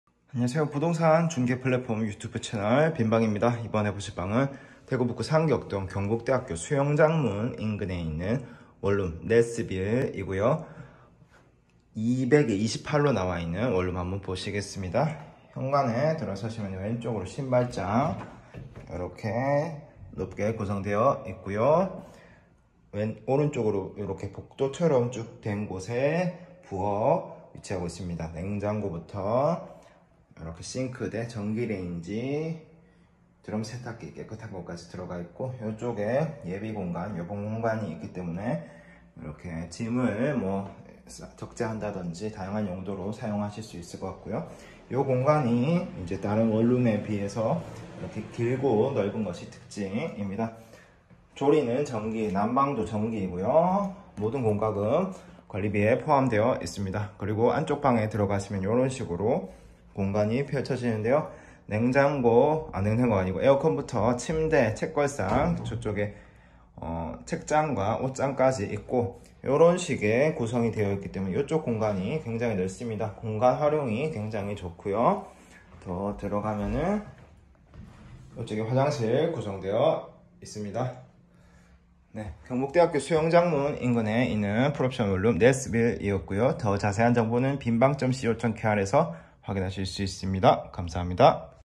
산격동 경북대 수영장문 인근 원룸 네스빌2
에서 '네스빌2'을 검색해 보세요.

대구 북구 산격동
비슷한 스펙의 학교주변 원룸과 비교해서
넓은 공간을 지닌 풀옵션 원룸
전기난방, 모든 공과금 포함